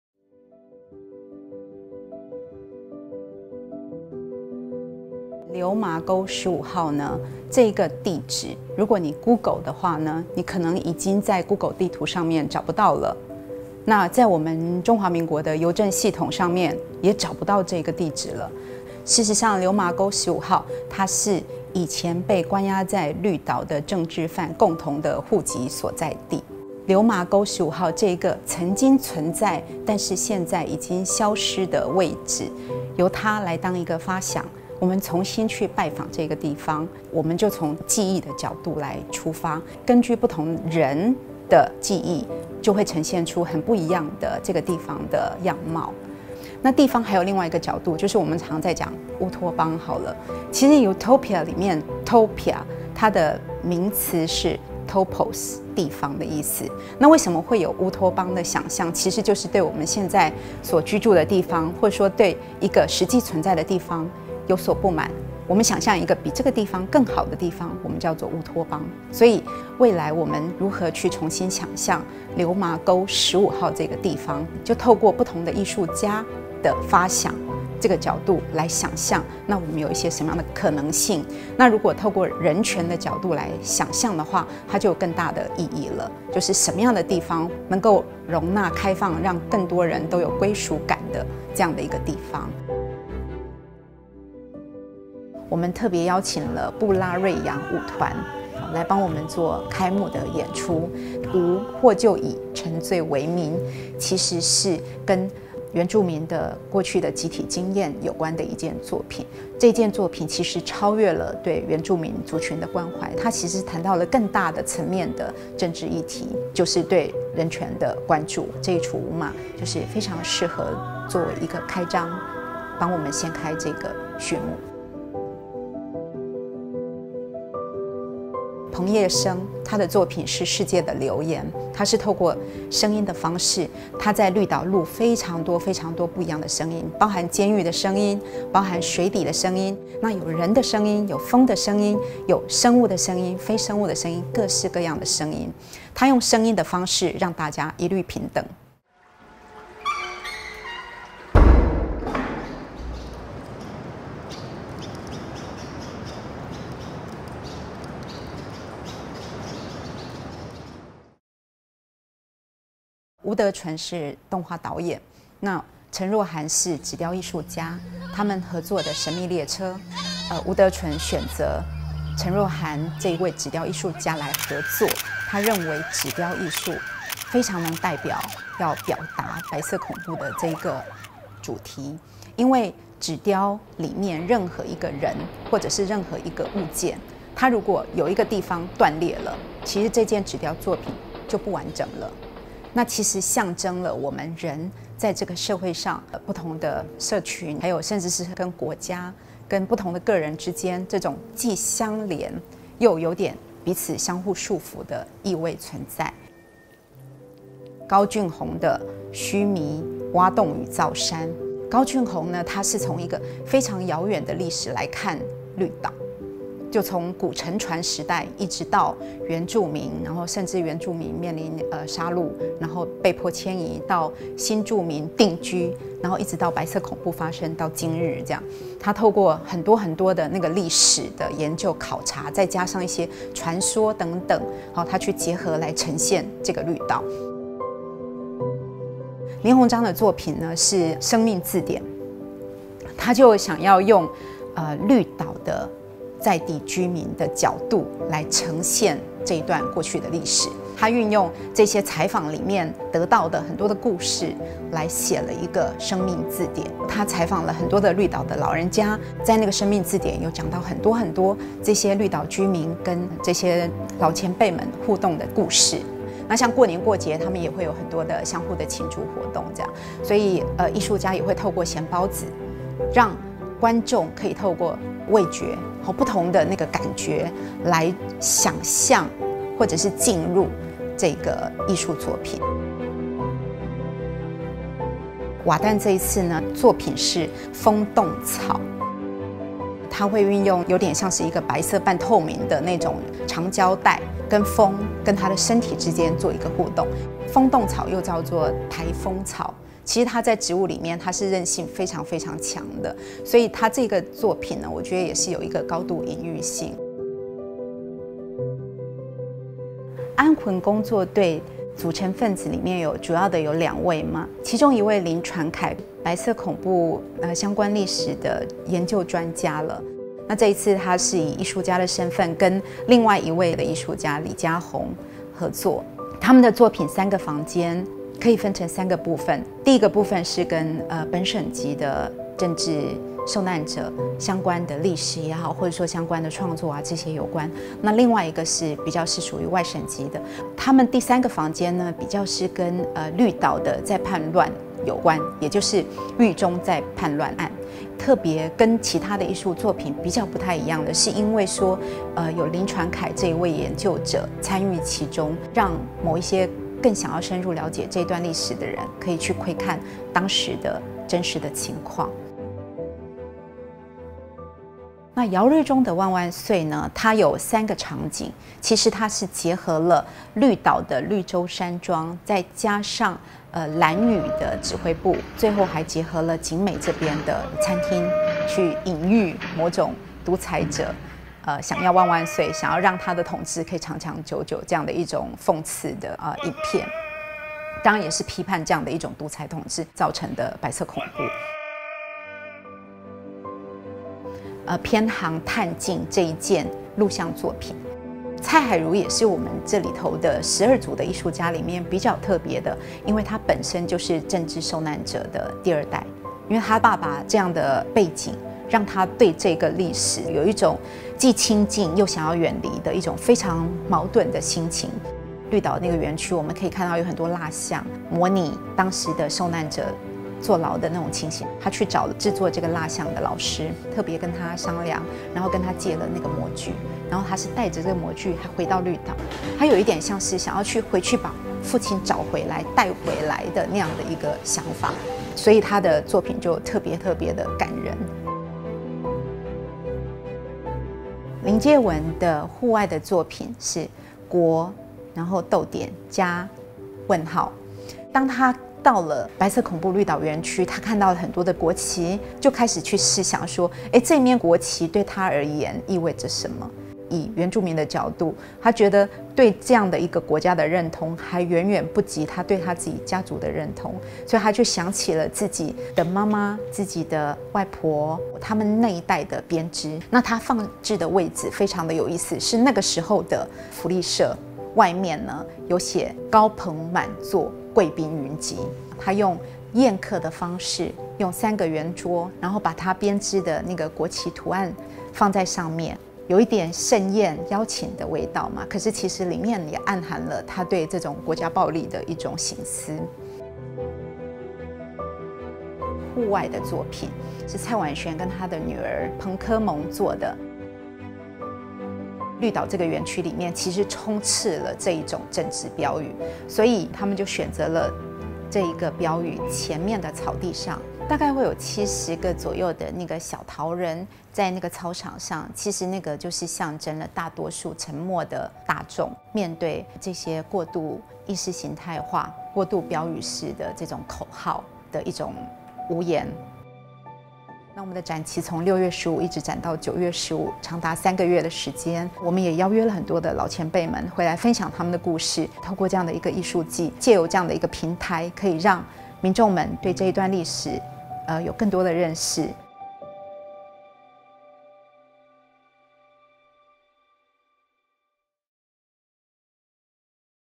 2019綠島人權藝術季拜訪流麻溝十五號 展覽預告片 2019 Green Island Human Rights Art Festival trailer
2019 綠島人權藝術季
2019 Green Island Human Rights Arts Festival

主辦單位：國家人權博物館

藝術家：林介文、林宏璋、成若涵、吳德淳、高俊宏、姚瑞中、瓦旦．塢瑪、蔡宛璇/澎科萌、蔡海如、澎葉生、布拉瑞揚舞團、安魂工作隊

策展人：羅秀芝
協同策展人：沈菲比

Supervisor：The Ministry of Culture of Taiwan
Organizer：National Human Rights Museum

Artists：Gieh-Wen Lin , Hong-John Lin, Jo-Han Cheng, De-Chuen Wu , Jun-Honn Kao , Jui-Chung Yao , Watan Uma , Wan-Shuen Tsai/ Lysianassa Dauby , Hai-Ru Tsai, Yannick Dauby,
Bulareyaung Dance Company、Libera work-gang

Curator：Sandy Hsiu-Chih Lo
Co-curator：Phebea Chun-Yi Shen